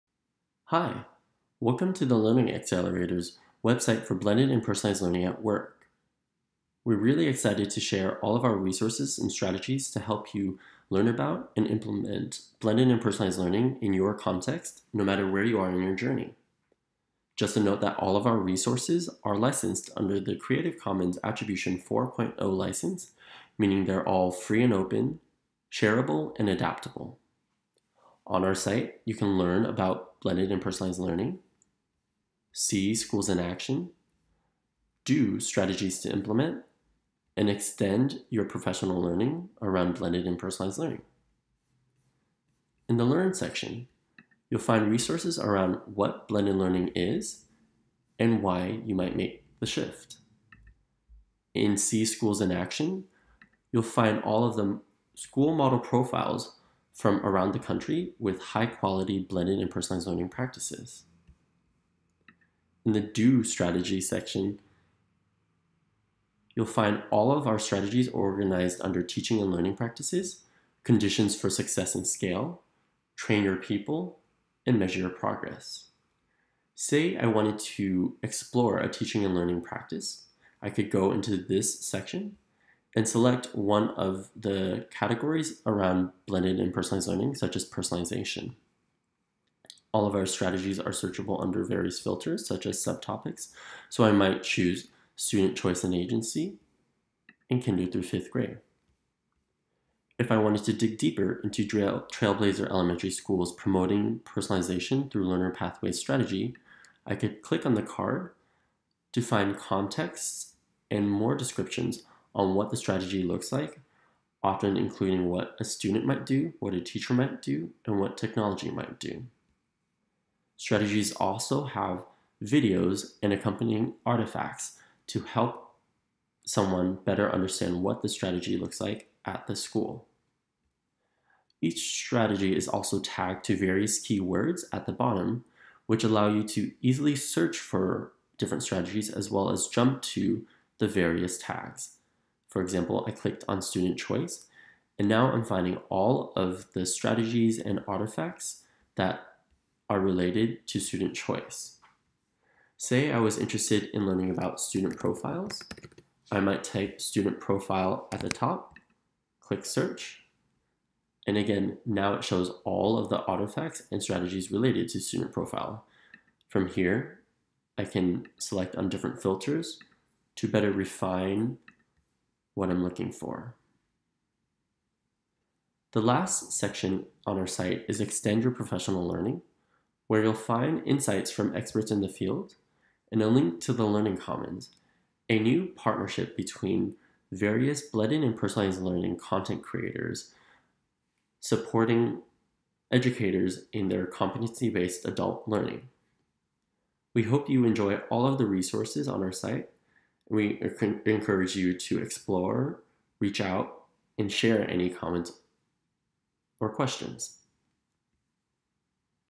New User Guide - Blended and Personalized Learning At Work Website
Here is a quick video from Stephen Pham, an Associate Partner at TLA, to show you how to access our refreshed Blended and Personalized Learning At Work site and content in some fun ways.